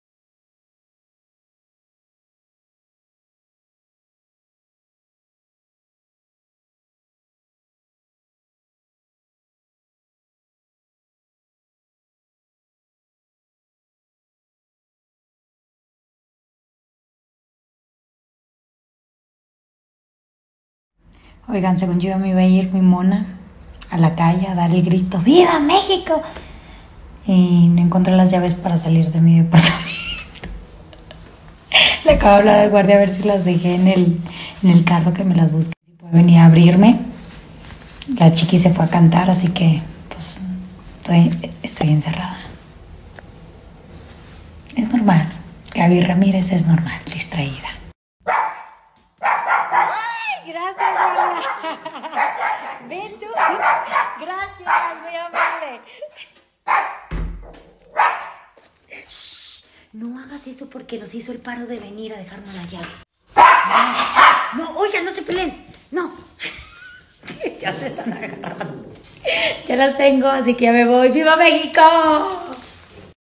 Gaby se quedó encerrada en su departamento al perder las llaves de la puerta el 15 de septiembre
15 SEPTIEMBRE